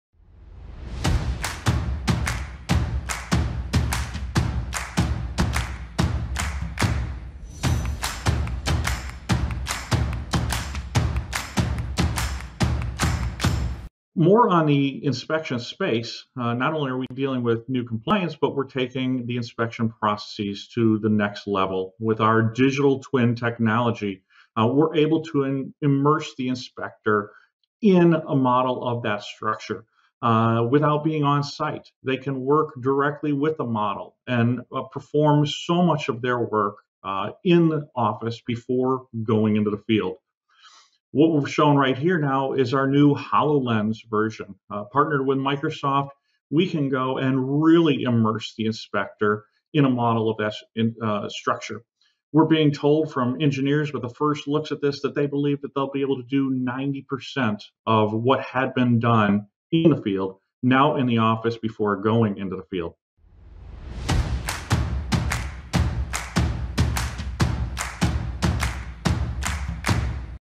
Immersive Bridge Inspections with AssetWise
Imagine if 90% of bridge inspections are done in the office? Now you can through immersive virtual inspection capabilities with AssetWise and Microsoft HoloLens, an inspector could conduct 90% of the inspection in the office reducing time in the field, making the inspection process quicker and safer and meet compliance.